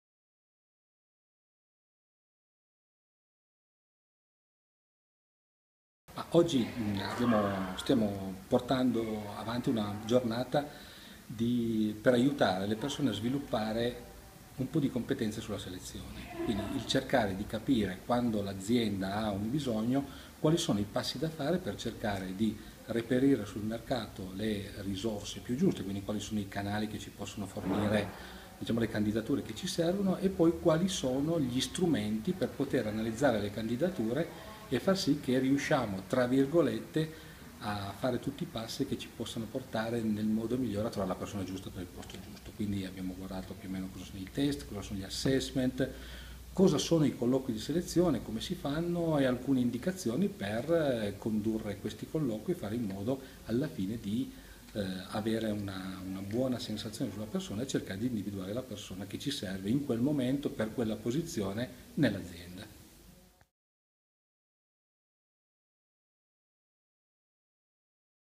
La selezione del personale: interviste, questionari e assessment
L'intervento consente, a chi ha già una conoscenza di base sulla selezione e valutazione del personale, di familiarizzare con strumenti di analisi del profilo personale ("Big Five Questionnaire") e sperimentare la metodologia dell'assessment center.